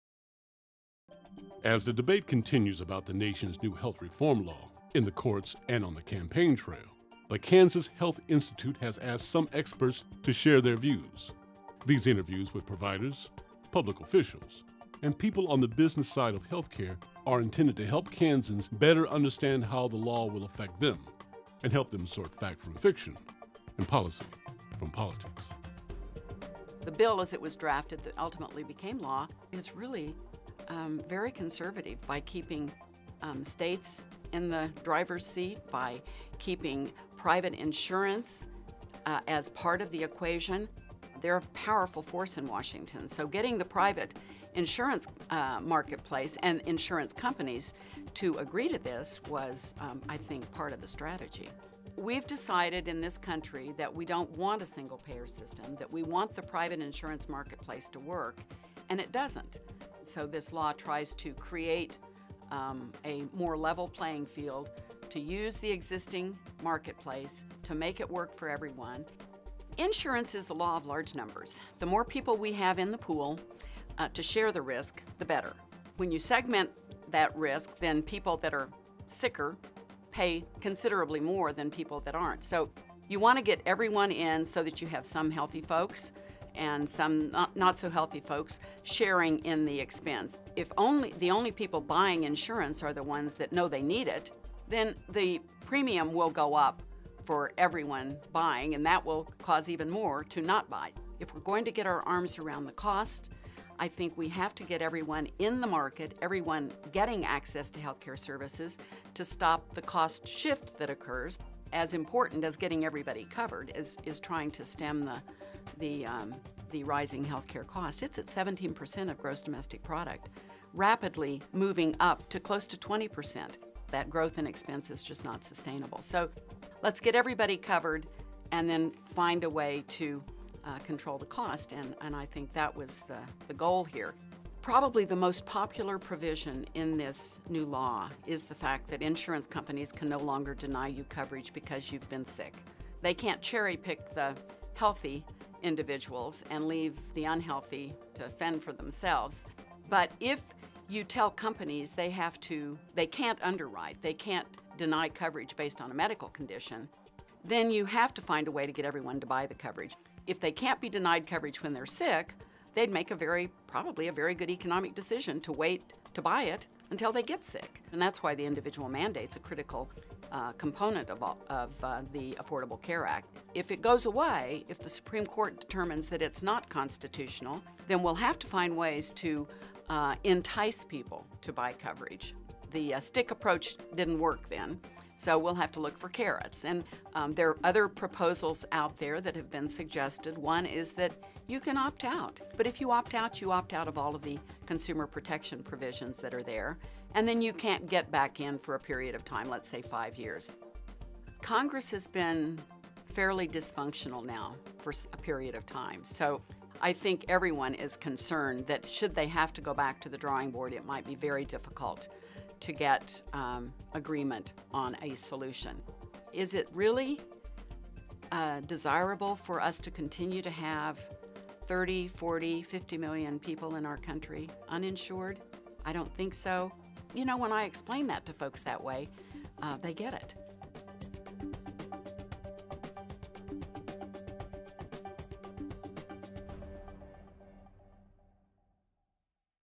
The Great Health Reform Debate: Sandy Praeger, Kansas Insurance Commissioner
Public opinion remains equally split on the Affordable Care Act. The latest monthly tracking poll by the Kaiser Family Foundation shows that 42 percent of Americans hold a favorable opinion of the law while 43 percent oppose it.  Those numbers have not changed much in months.

To help Kansans better understand the law, the problems it was intended to address and the issues that prompted Kansas and 25 other states to challenge it in court, the Kansas Health Institute recently talked to several experts. Excerpts from those conversations are featured in this collection of short videos.  Each offers an informed perspective on the law and the controversy that surrounds it as the U.S. Supreme Court prepares to rule on its constitutionality.

This is one of six short videos.